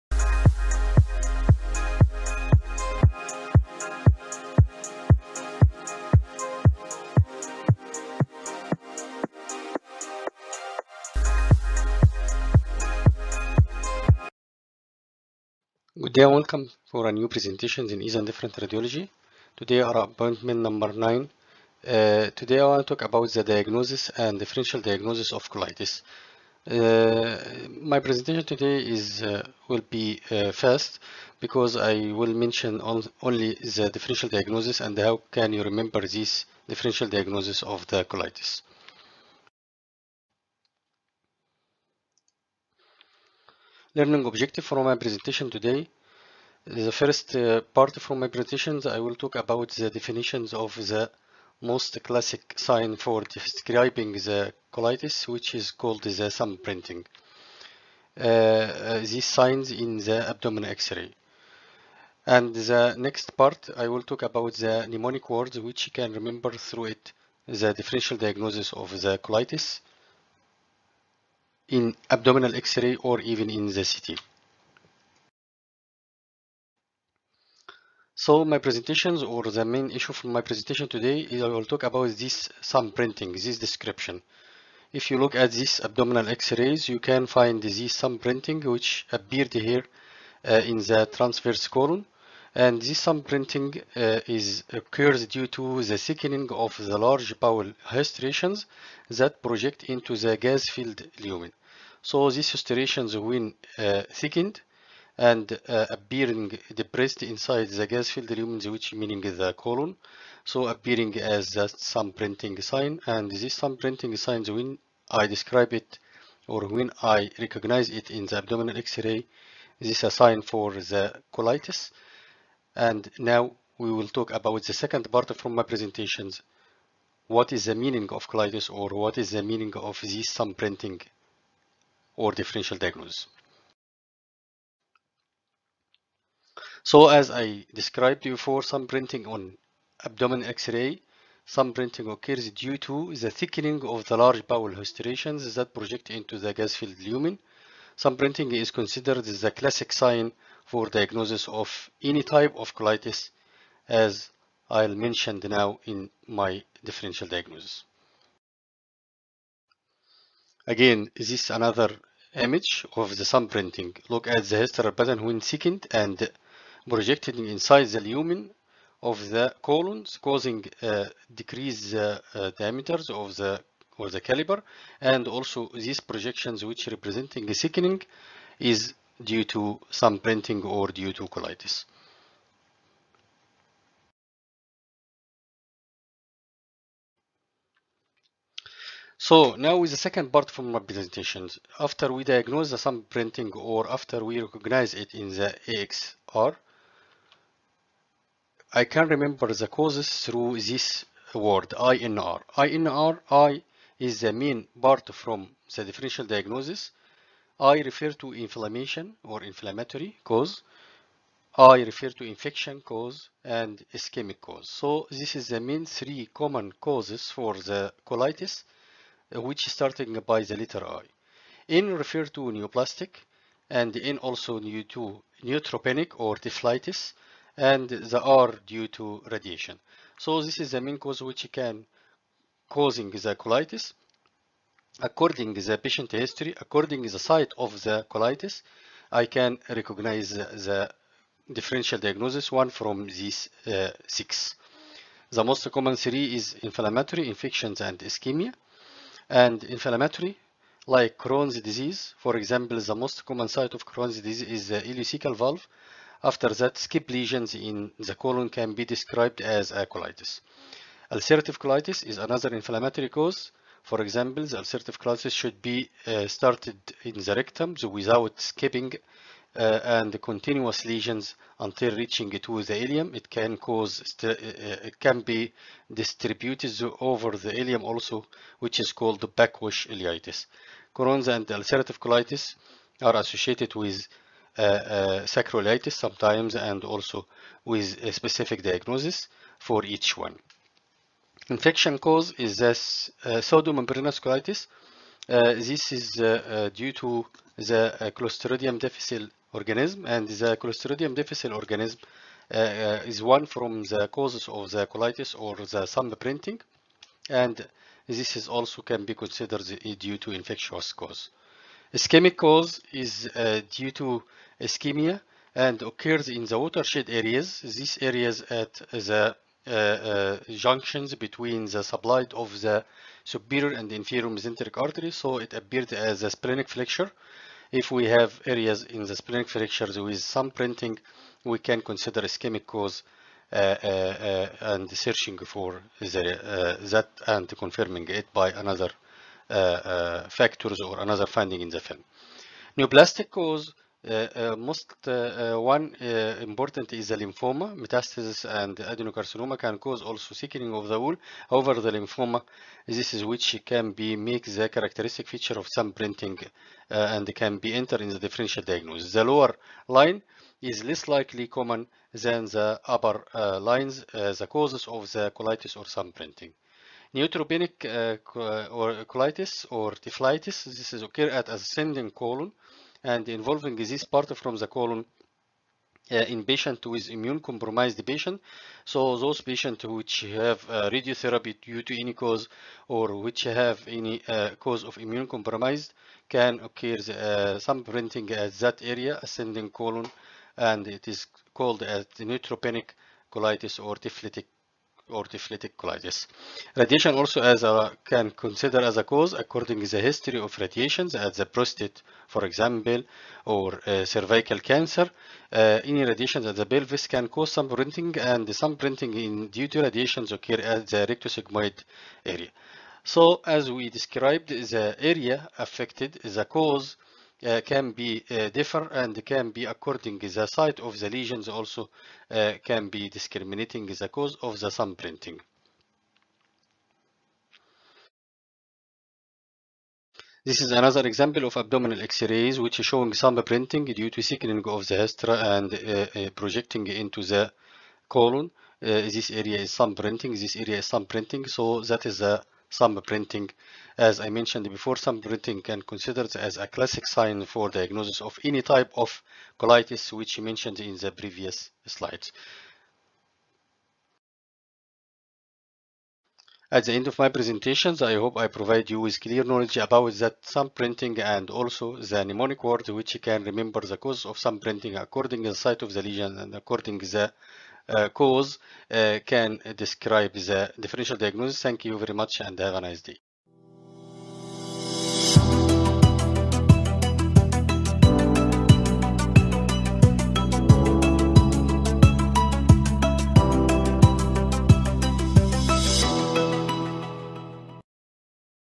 Colitis and thumbprinting (Radiology in GIT- 9)
On AXR, thumbprinting occurs due to the thickening of large bowel haustrations that project into the gas-filled lumen
Thumbprinting is considered the classic sign for diagnosis of any type of colitis